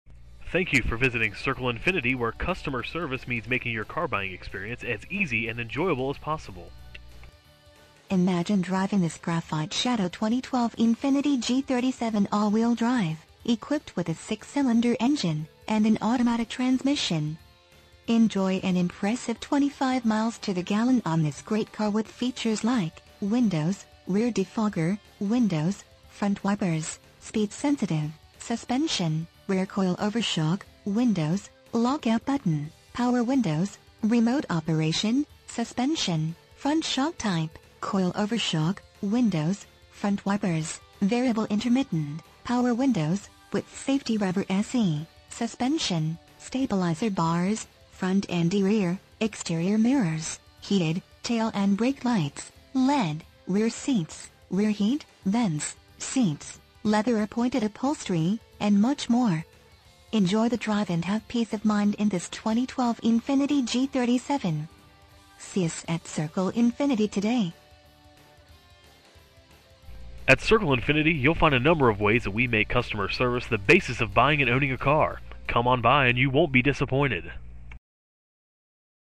2012 Infiniti G37 Sedan Circle Infiniti West Long Branch, NJ 07764
Infiniti G Sedan Imagine driving this Graphite Shadow Infiniti G37 Sedan 4 Dr Sedan AWD, equipped with a 6 Cylinder engine and an automatic transmission. Enjoy an impressive 25 miles to the gallon on this great car with features like Windows, Rear Defogger, Windows, Front Wipers: Speed Sensitive, Suspension, Rear Coil Over Shock, Windows, Lockout Button, Power Windows: Remote Operation, Suspension, Front Shock Type: Coil Over Shock, Windows, Front Wipers: Variable Intermittent, Power Windows: With Safety Reverse, Suspension, Stabilizer Bars: Front And Rear, Exterior Mirrors, Heated, Tail And Brake Lights, LED, Rear Seats, Rear Heat: Vents, Seats, Leather-Appointed Upholstery, Seats, Front Seat Type: Bucket, Fixed Rear Headrests, Security, Anti-Theft Alarm System With Engine Immobilizer, One-Touch Windows: 4, Warnings And Reminders, Maintenance Reminder, Rear Seats, Center Armrest: Folding With Pass-Thru, Front Fog Lights, and much more. Enjoy the drive and have peace of mind in this Infiniti G37 Sedan. See us at Circle Infiniti today!
MILES:  6,128
VIN:JN1CV6AR4CM976703

Circle Infiniti
301 Highway 36
West Long Branch, New Jersey 07764